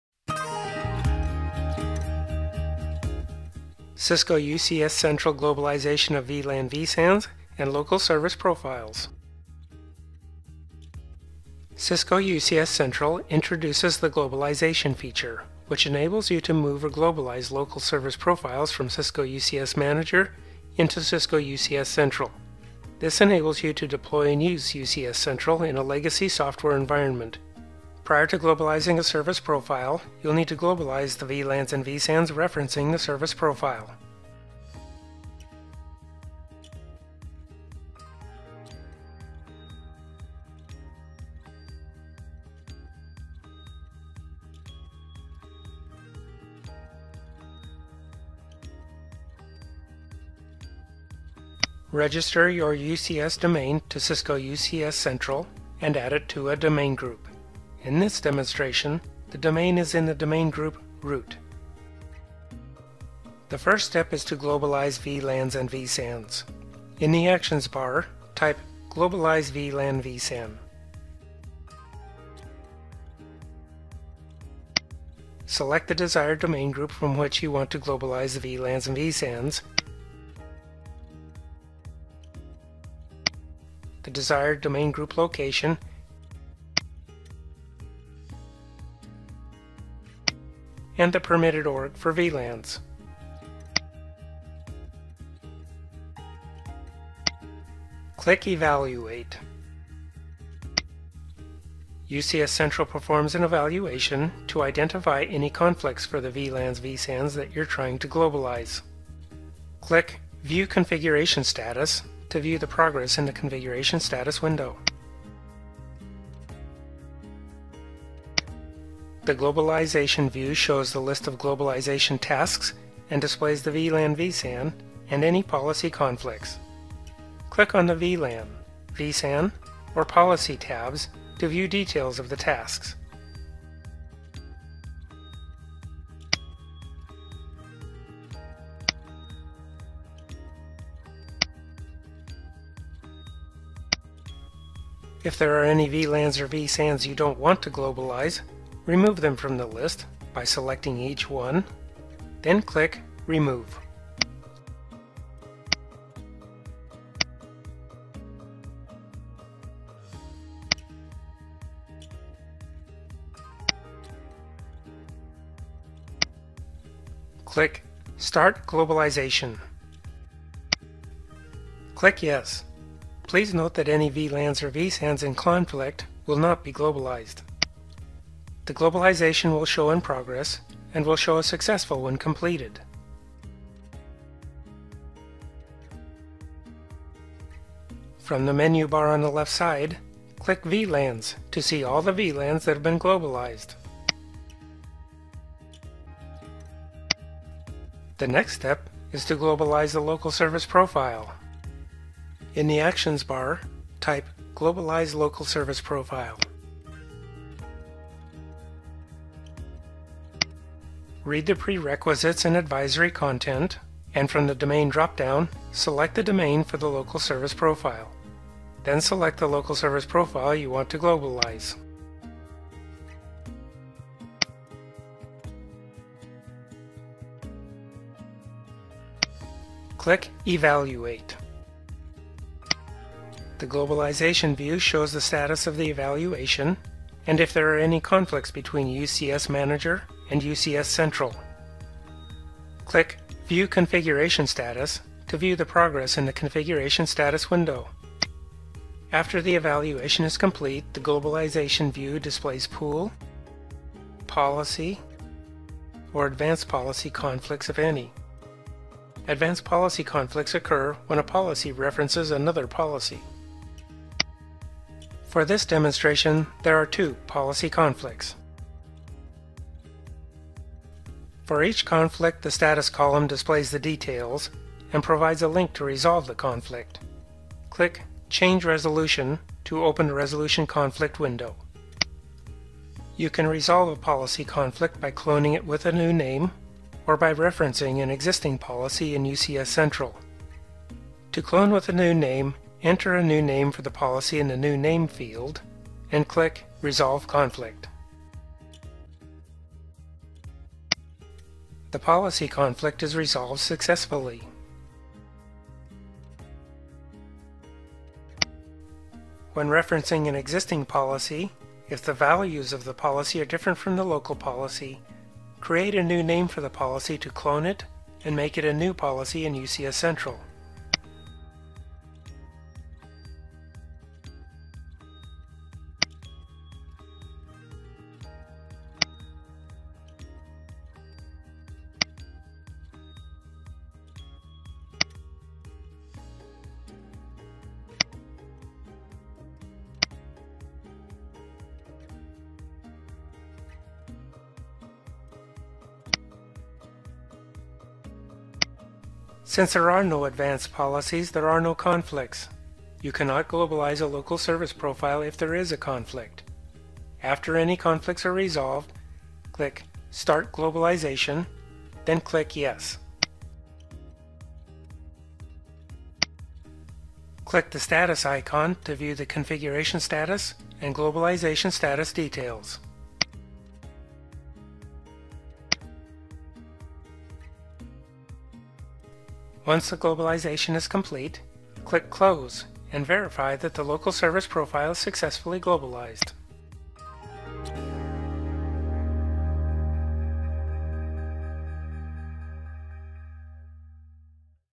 Globalize Local Service Profile and VLAN/VSAN
This video shows you how to migrate existing Service Profiles from UCS Manager into UCS Central, which enables you to deploy and use Cisco UCS Central in a legacy software environment. Once globalized, you can then take full advantage of all of the optimizations and capabilities of UCS Central. The simple click-through process allows for orderly and non-disruptive migration of VLANs, VSANs, Service Profiles and Policies.